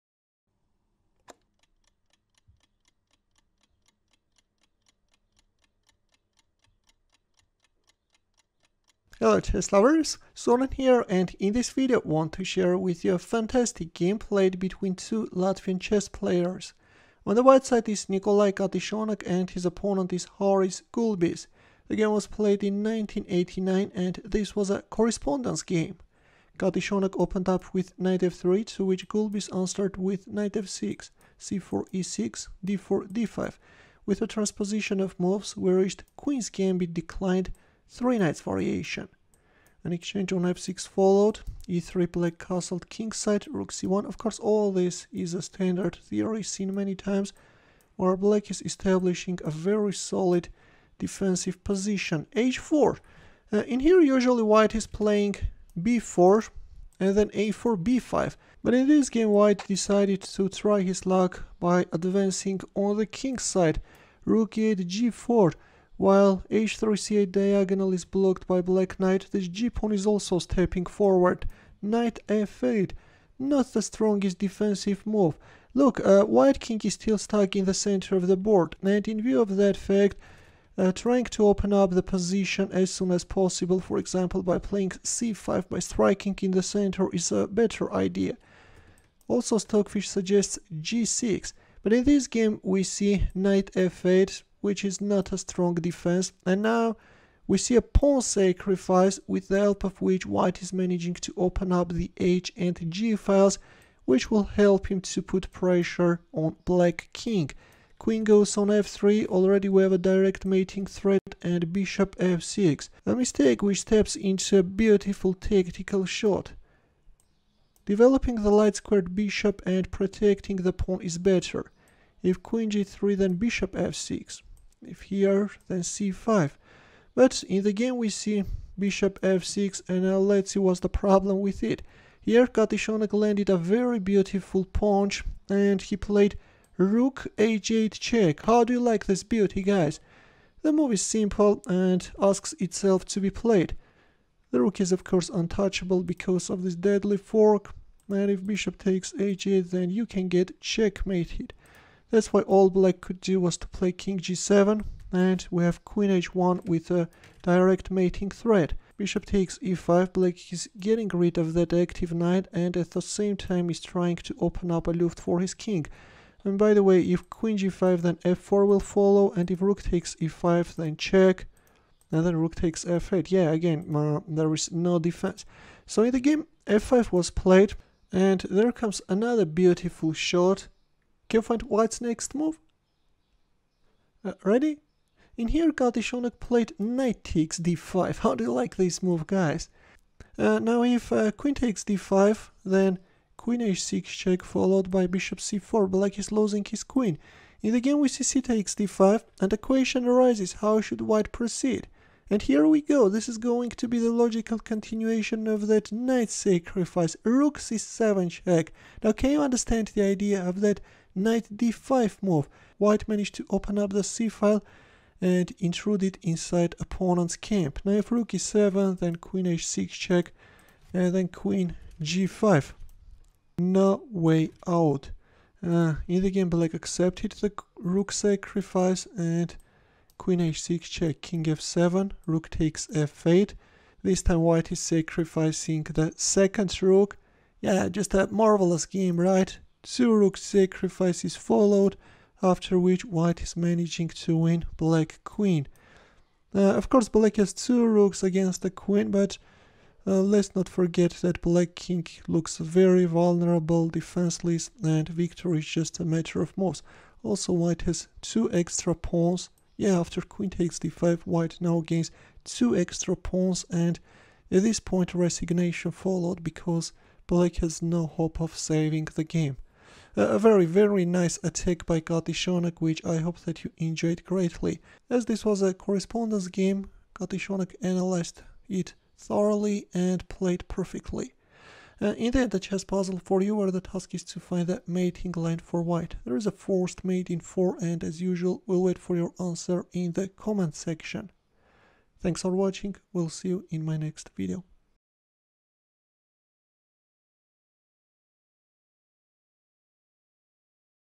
Latvian Chess Player Sacrifices Pieces In The Style Of His Compatriot
Nikolaj Katishonok vs Harijs Gulbis
 USSR (1989) (correspondence)
Queen's Gambit Declined: Three Knights Variation. General (D37)
1.Nf3 Nf6 2.c4 e6 3.d4 d5 4.Nc3 Be7 5.Bg5 h6 6.Bxf6 Bxf6 7.e3
O-O 8.Rc1 c6 9.Bd3 Nd7 10.cxd5 exd5 11.h4 Re8 12.g4 Nf8 13.g5
hxg5 14.hxg5 Bxg5 15.Ne5 g6 16.Qf3 Bf6 17.Rh8+ Kg7 18.Qh1 Bxe5
19.dxe5 f5 20.Nxd5 cxd5 21.Rc7+ Qxc7 22.Qh6+ Kf7 23.Rxf8+ Rxf8
24.Qh7+ Ke8 25.Qxc7 Bd7 26.Qxb7 Ke7 27.Qxd5
1-0